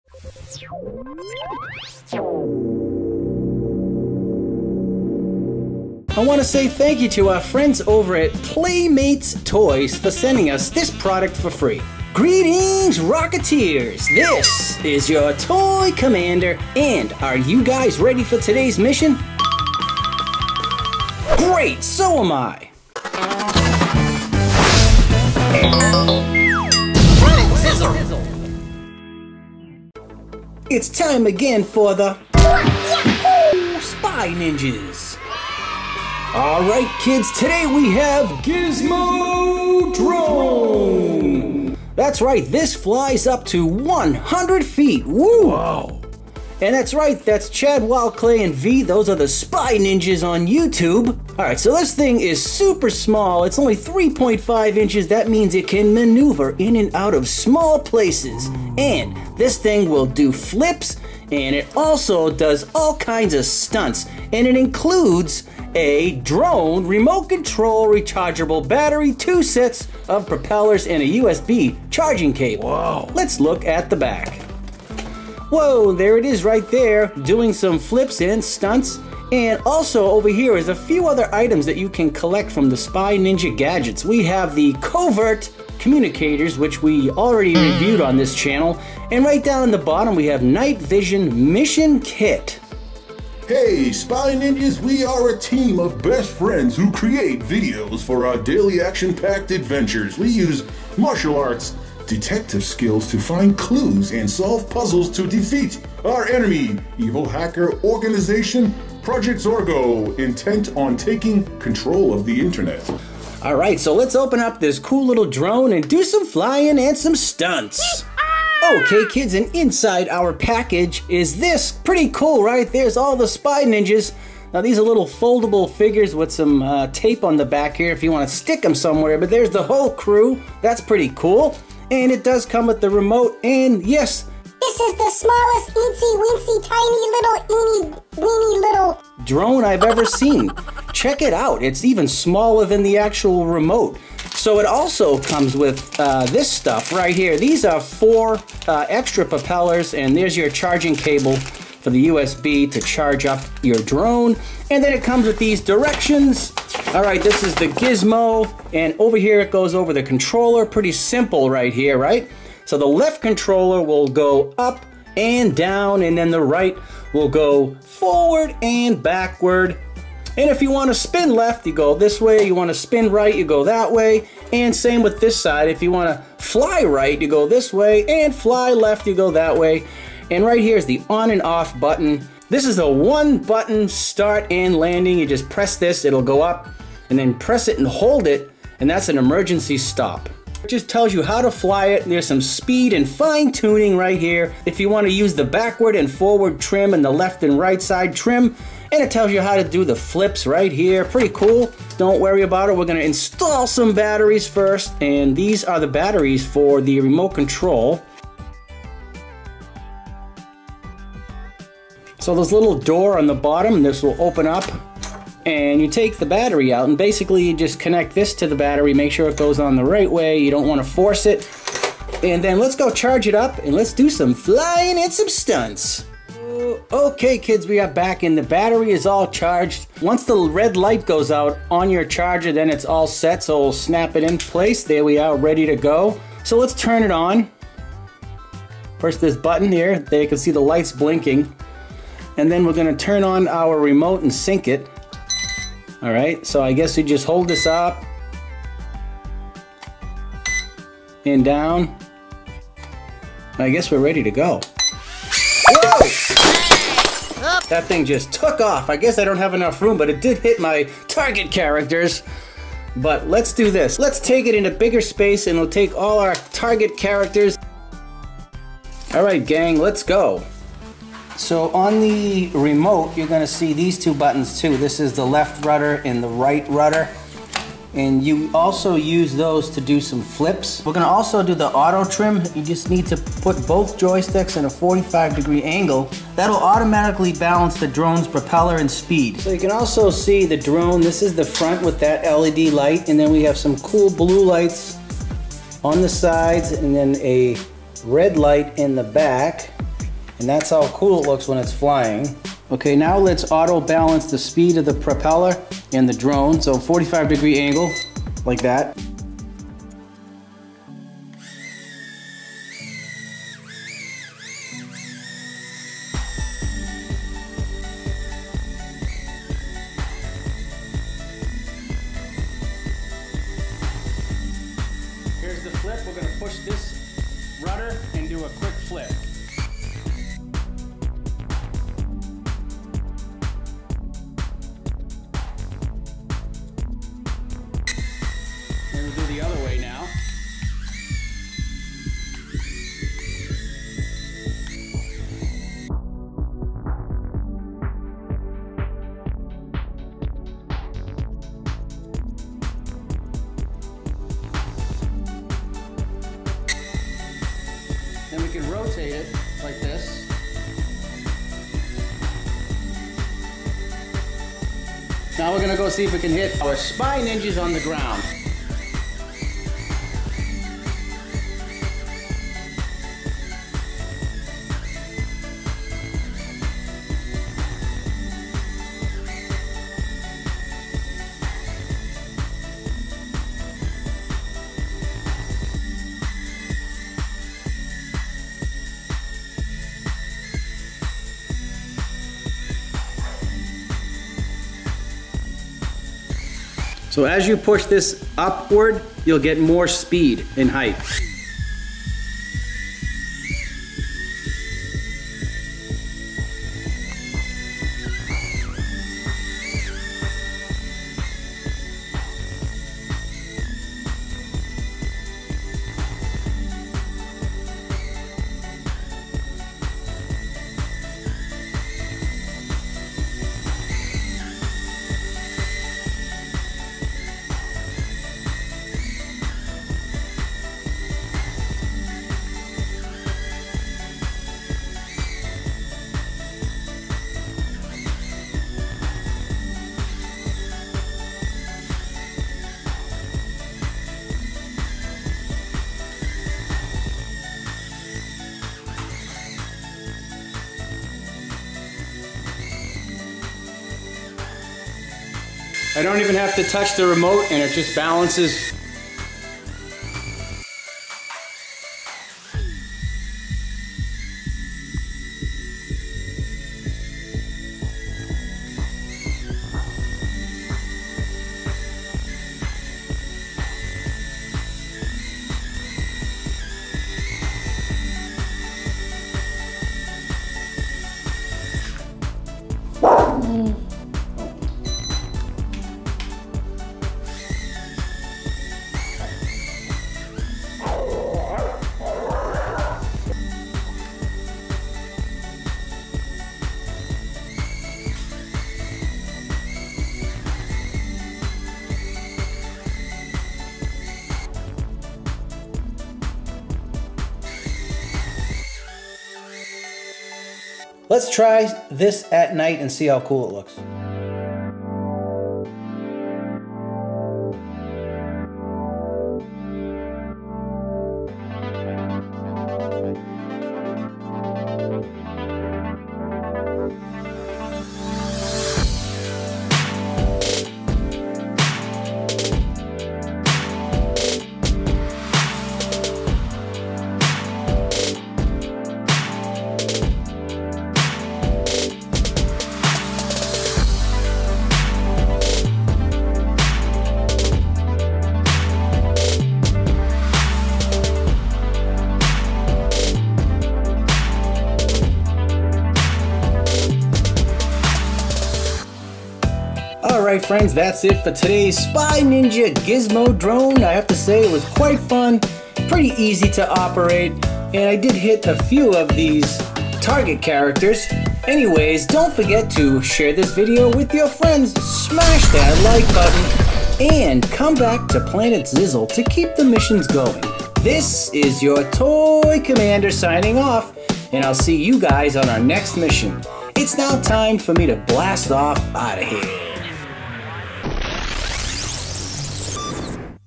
SPY NINJAS Gizmo Drone
Thank you to Playmates Toys for this free product.
Greetings Rocketteers, the Gizmo Drone has landed on Planet Zizzle.
Become a Gizmo drone pilot.  This drone includes remote control, rechargeable battery, 2 sets of propellers, USB charging cable.  Get ready for some flips, stunts and flies up to 100 feet and also includes target characters.